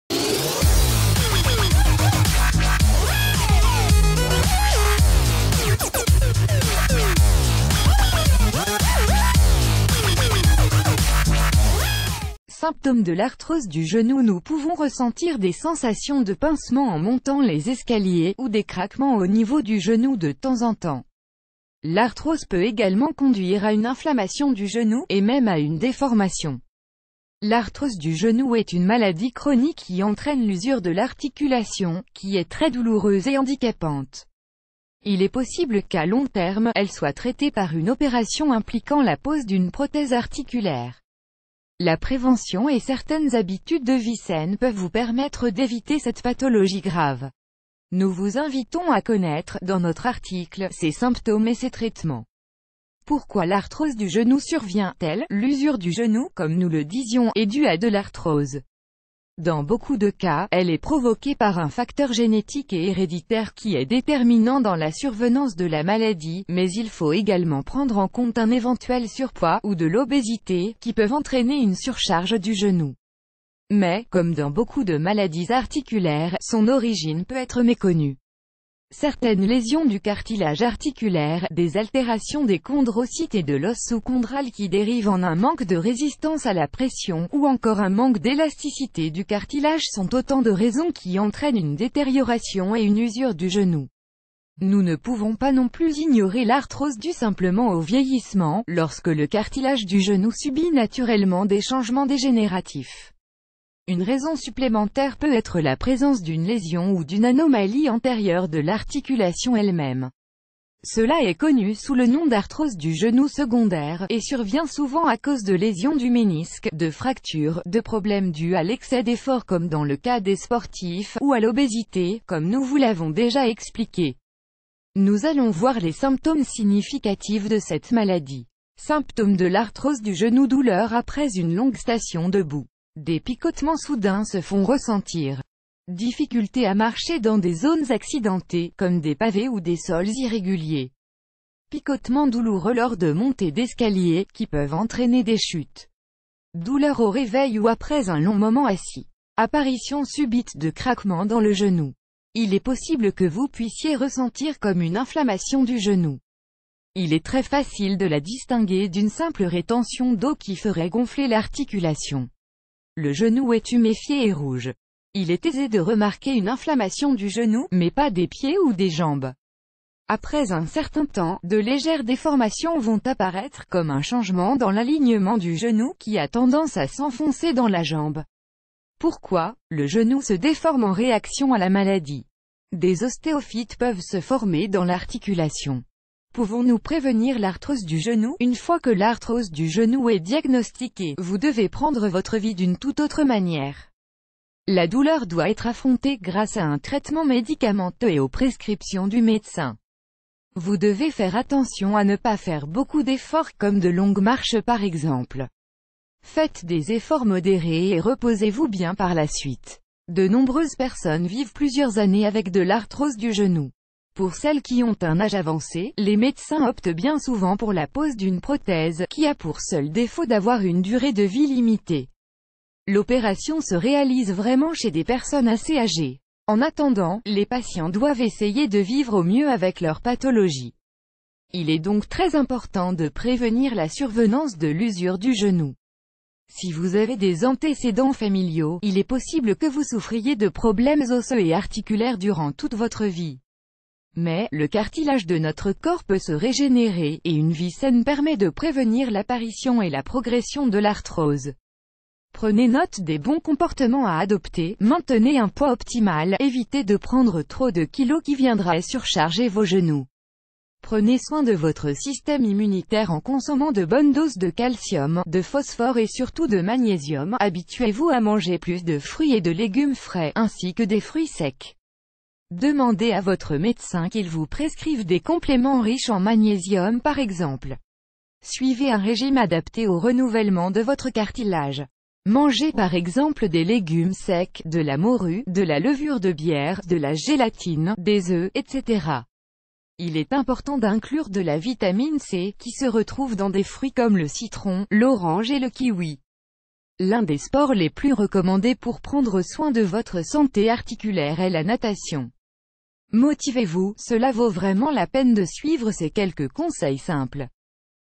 Symptômes de l’arthrose du genou
Symptômes de l’arthrose du genou Nous pouvons ressentir des sensations de pincement en montant les escaliers, ou des craquements au niveau du genou de temps en temps. L'arthrose peut également conduire à une inflammation du genou, et même à une déformation. L’arthrose du genou est une maladie chronique qui entraîne l’usure de l’articulation, qui est très douloureuse et handicapante. Il est possibl...